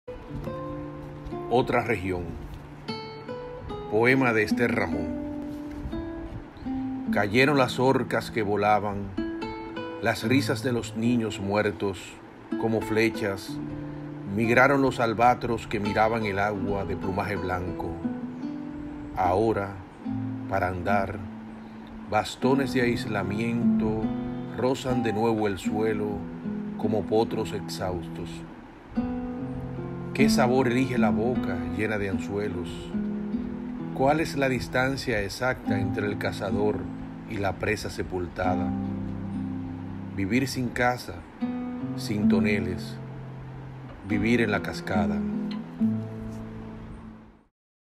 Otra región - Esther Ramón
Serie: Poemas leídos al azar en  tiempos de cuarentena
Poema de Esther Ramón

Voz: José Enrique Delmonte